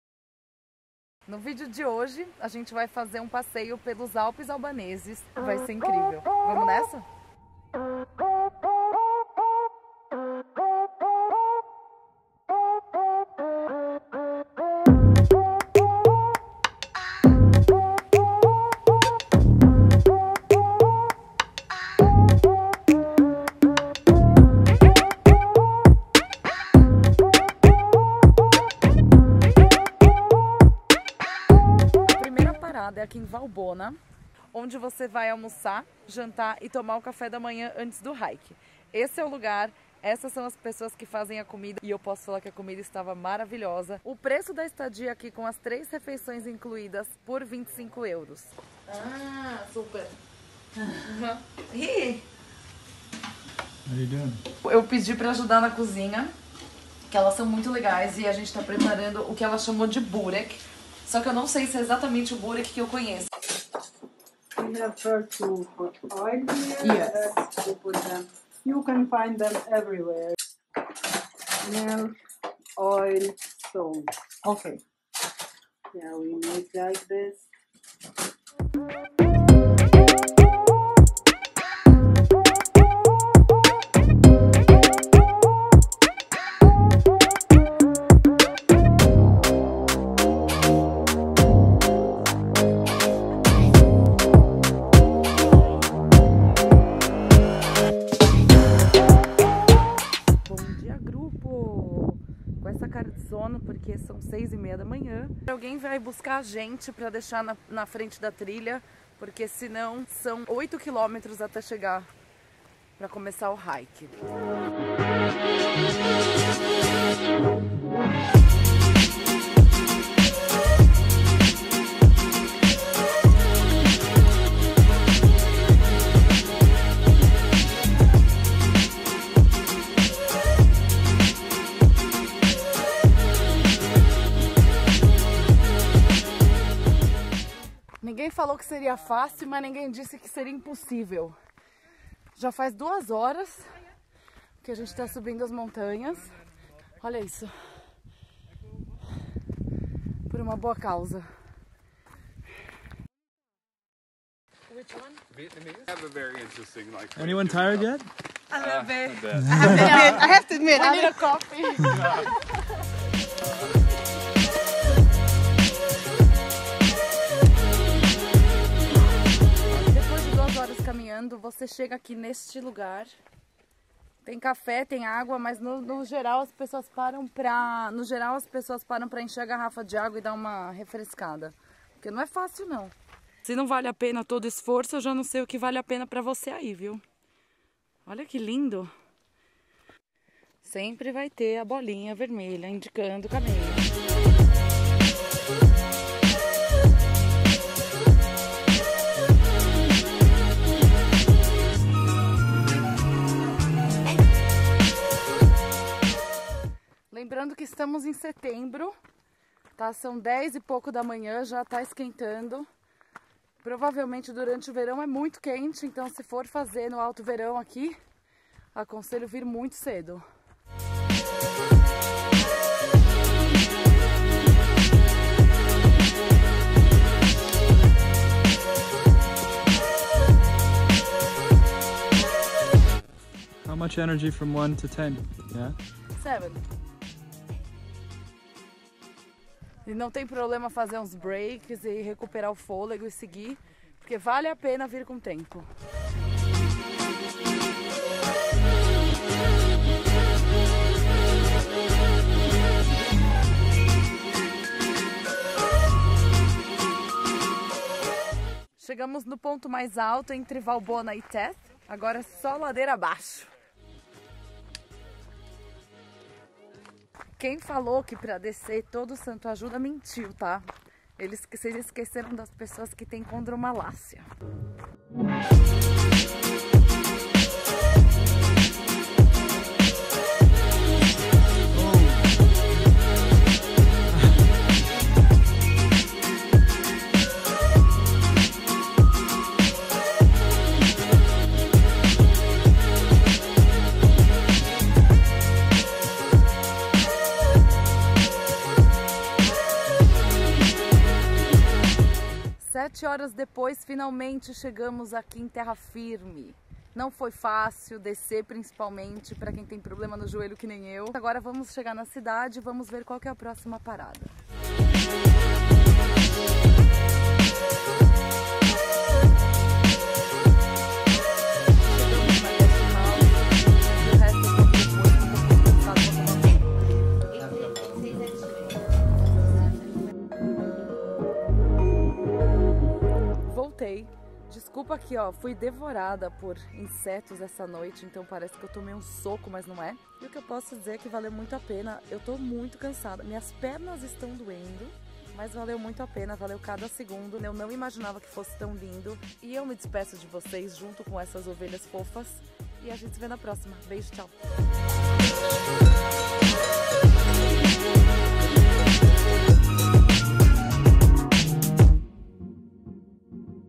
Hike nos Alpes da Albânia? SIM!
Alpes na Albânia? Sim, eles existem.

Além de ser um lugar surreal de bonito, você ainda pode fazer um hike incrível.A jornada começa em Shkoder, onde você vai descobrir as belezas do lago Komani, desembarcar em Valbona, vivenciar uma experiência transformadora até se desafiar por 12km na travessia até Theth.

Mas, não vou me estender muito.
Tire suas próprias conclusões hehehe

0:01 Ferry pelo Lago Komani
0:30 A experiência em Valbona antes do hike
1:55 A travessia
5:00 Por fim